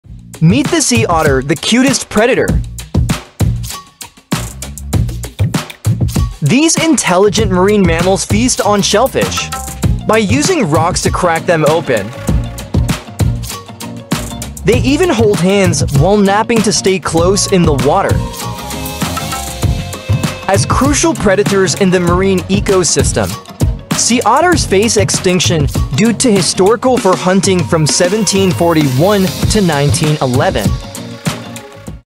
Adorable Predators: The Sea Otter's Clever Dining Habits
Explore the enchanting world of sea otters in this captivating video! Watch as these intelligent marine mammals showcase their clever dining habits, including using rocks to crack open shellfish. The heartwarming image of sea otters holding hands while napping adds to their charm. However, the video also sheds light on the grim reality they face, with the looming threat of extinction due to historical fur hunting. Join us on a journey through the secret lives of these adorable predators and the challenges they endure for survival.
Sea otter, intelligent, marine mammals, shellfish, rocks, dining habits, holding hands, napping, extinction, fur hunting, survival.